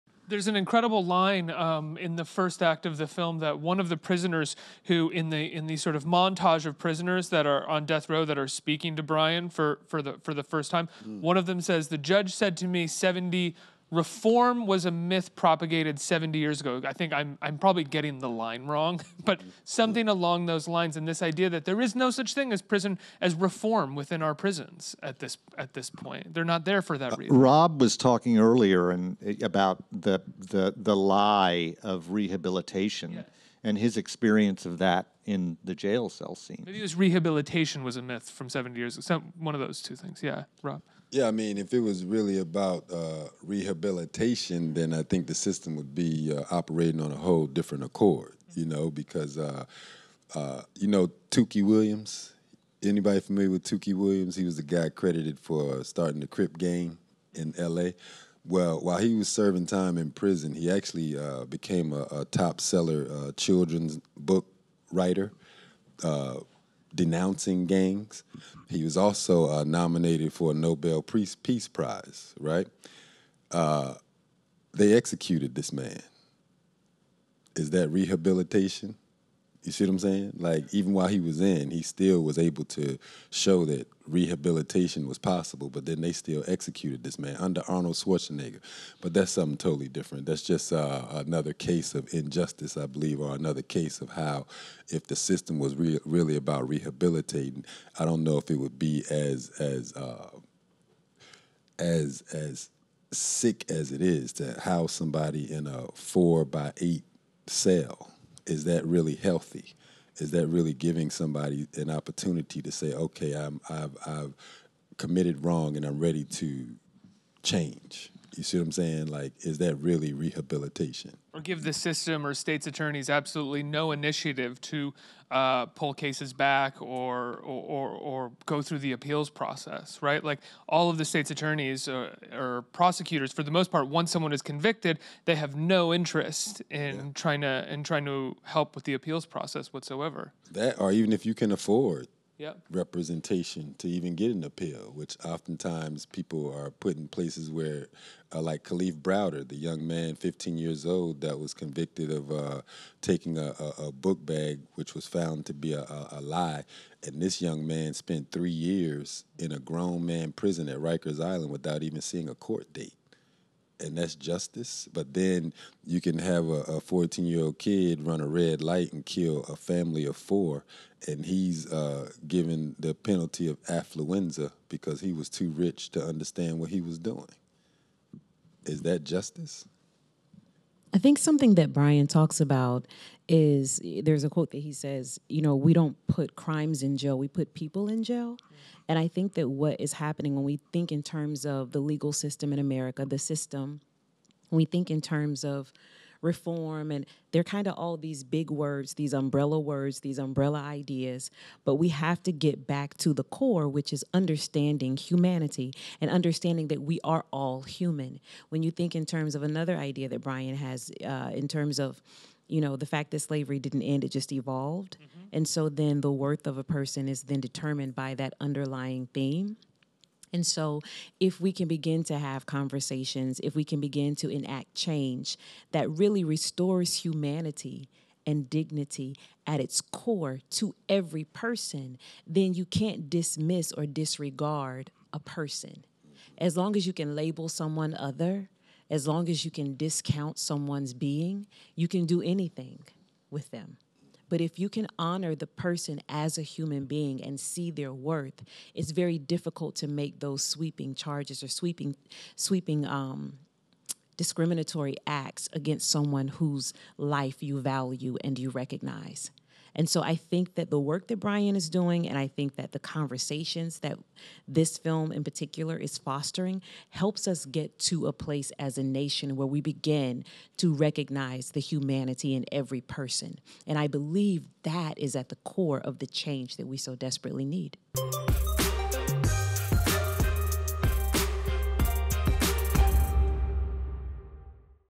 "Just Mercy" Actors Rob Morgan & Karan Kendrick Weigh In On The Flawed American Justice System
Rob Morgan and Karan Kendrick, stars in the legal drama, "Just Mercy," discuss the racism and corruption ingrained in the American justice system.